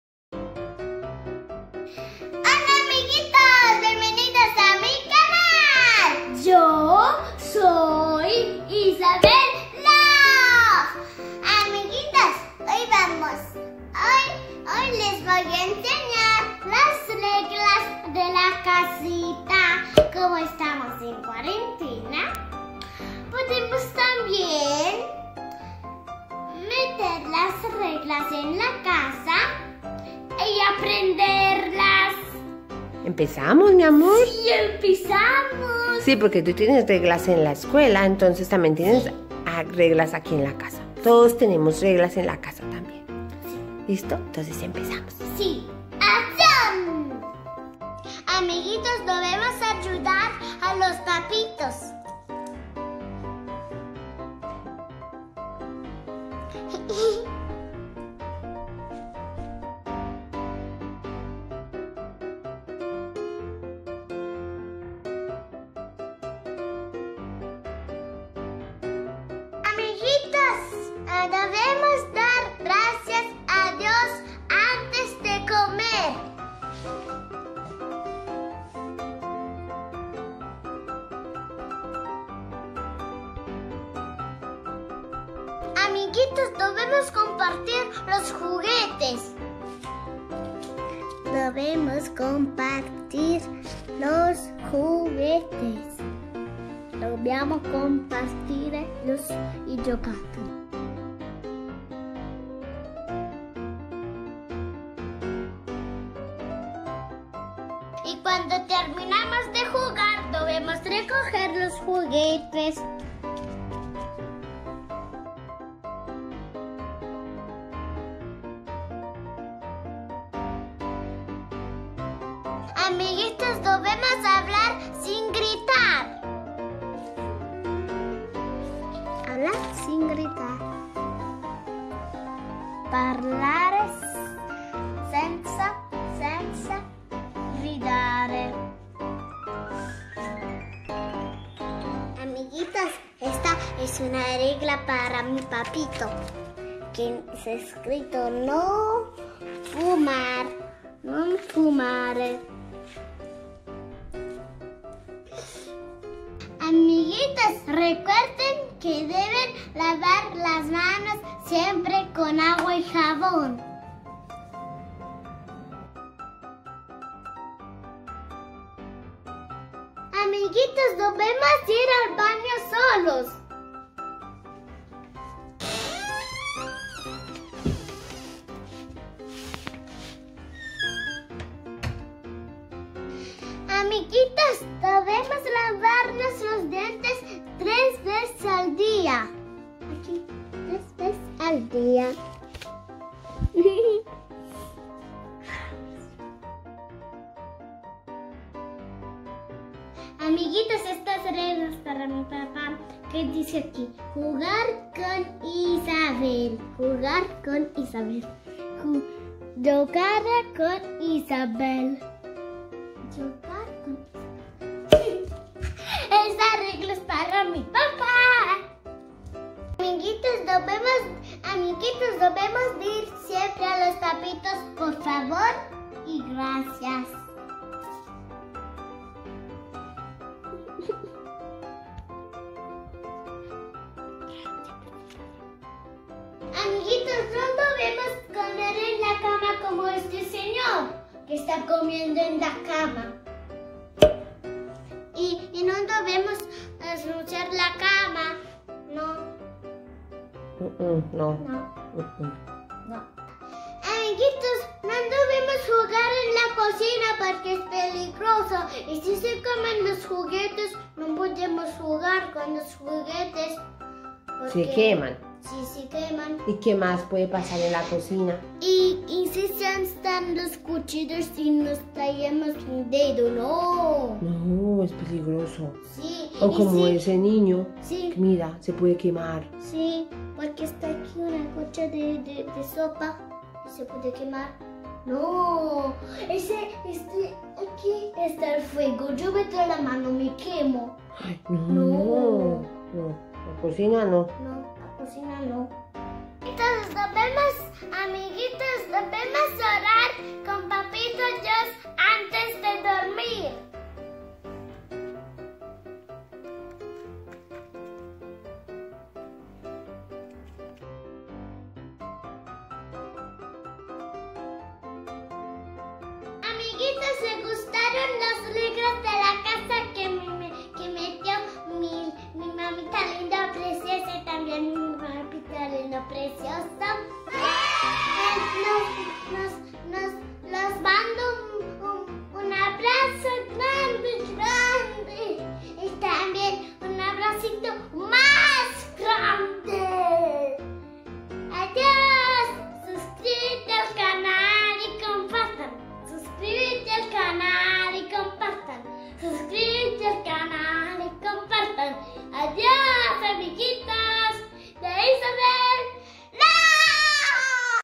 REGLAS PARA NIÑOS EN CASA|Videos educativos para niños
Las Reglas para niños en casa | Videos educativos para niños

Amiguitos  hoy les comparto las reglas de mi casa que mi mami coloco ahorita en la cuarentena 😅 Tambien les dejo otro enlace de un video anterior donde tambien comparto otros deberes en casa y cosas  que no debemos hacer ,que Dios Todopoderoso los proteja ,los quiero muchoteee

Ayudo a mis padres
Dar gracias a Dios antes de comer
Recojo mi habitación
Comparto mis juguetes
Recojer los juguetes
Pido las cosas por favor
Hablar sin gritar
No fumar ( los adultos )
Bañarse siempre las manos
Ir solos al baño
Lavarse los dientes tres veces al dia
Jugar con nuestros hijos
Decir por favor y gracias
No comer en la cama
No debemos jugar en la cocina
Orar antes de dormir
Saludar y despedirse siempre

💛💙❤️ Besitos a todos.